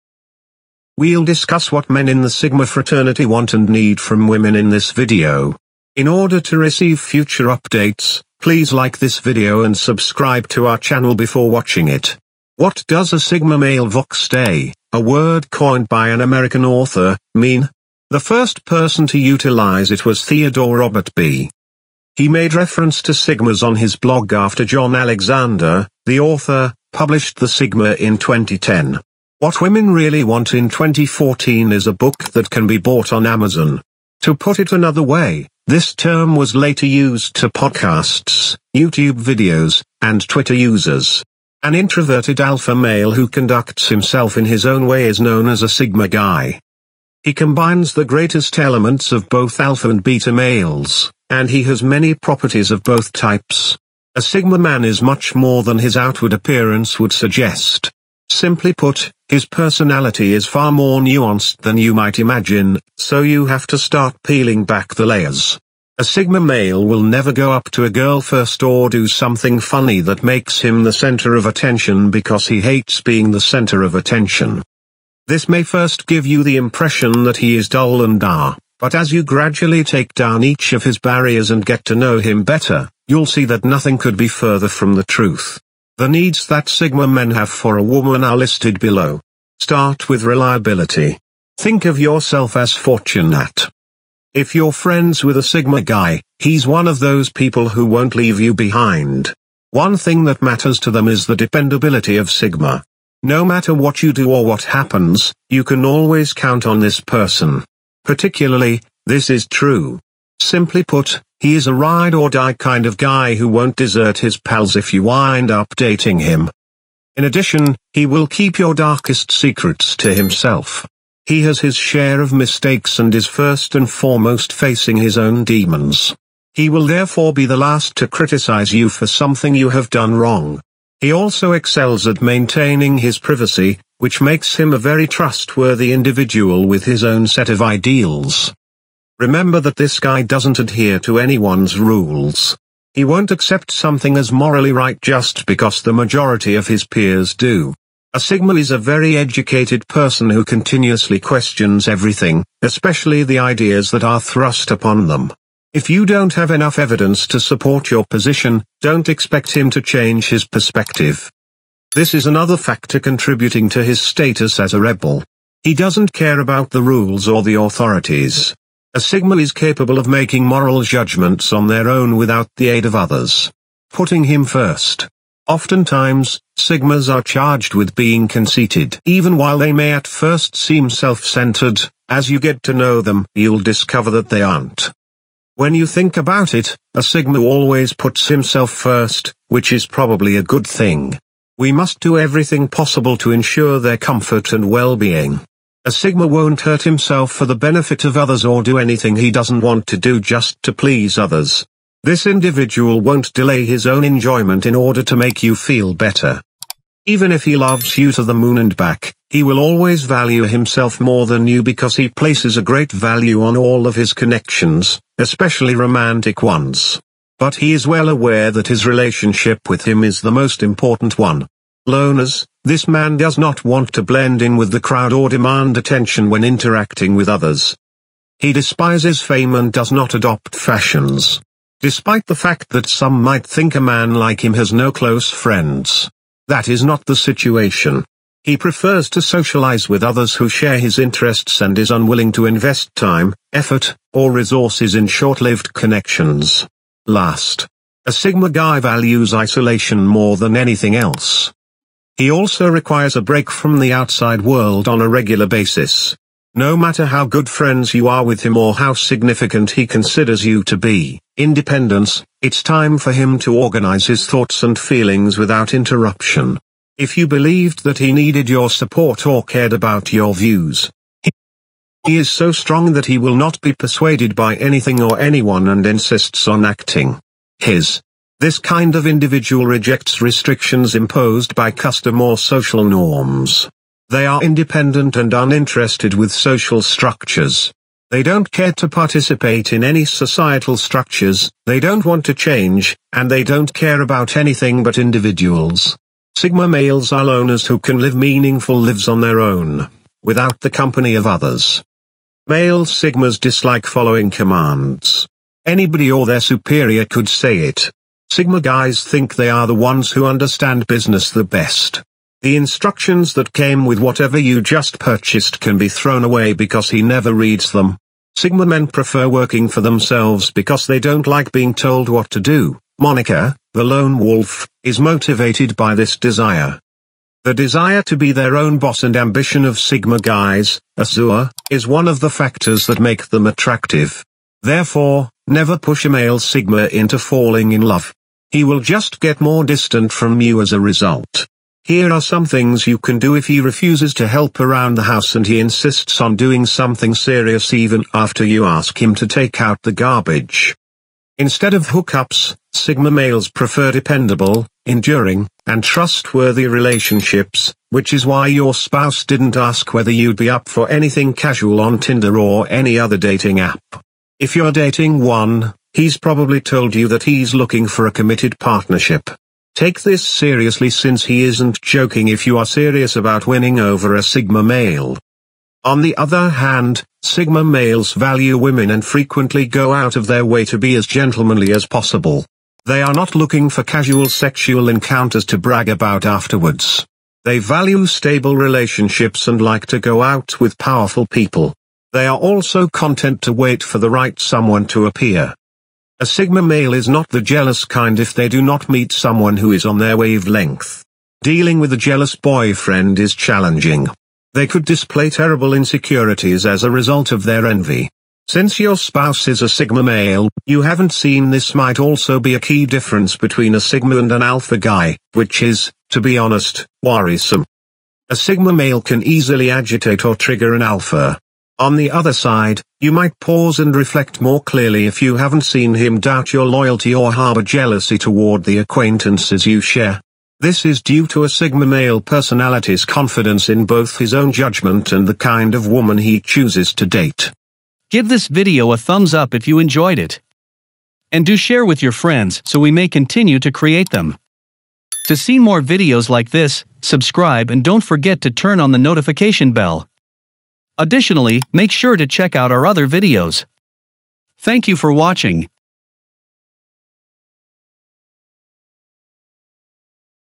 What Sigma Males NEED From a Woman - Sigma Male Wise Thinker
Script:
We'll discuss what men in the Sigma fraternity want and need from women in this video. What does a Sigma male Vox day—a word coined by an American author—mean? The first person to utilize it was Theodore Robert Be.

He made reference to sigmas on his blog after John Alexander, the author, published the Sigma in 2010. What Women Really Want in 2014 is a book that can be bought on Amazon. To put it another way, this term was later used to podcasts, YouTube videos, and Twitter users. An introverted alpha male who conducts himself in his own way is known as a "Sigma guy."